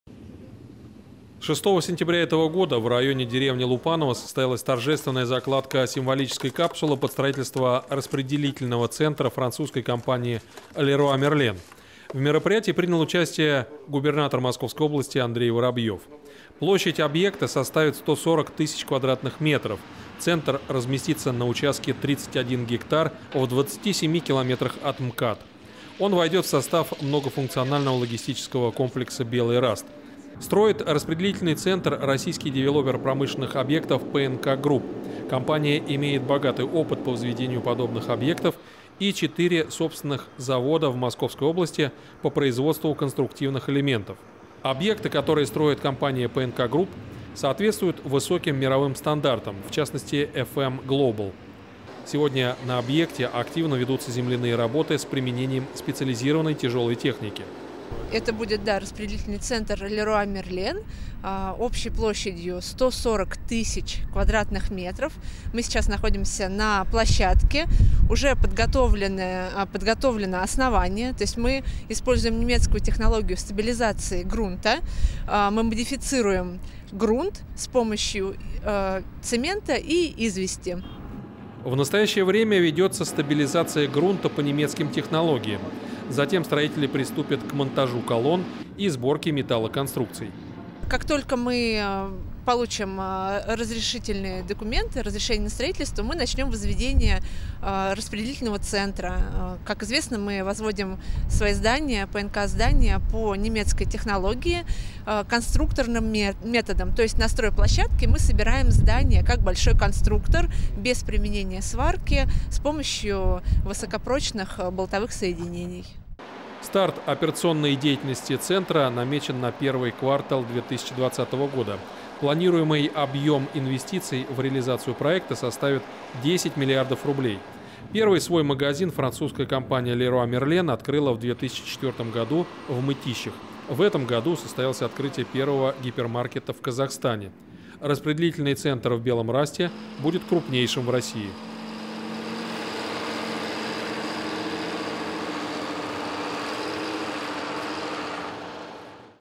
Строительство логистического комплекса «Леруа Мерлен»
Сегодня наш корреспондент побывал в окрестностях деревни Лупаново, где в сентябре началось строительство логистического  комплекса «Леруа Мерлен». Этот распределительный центр  будет крупнейшим в России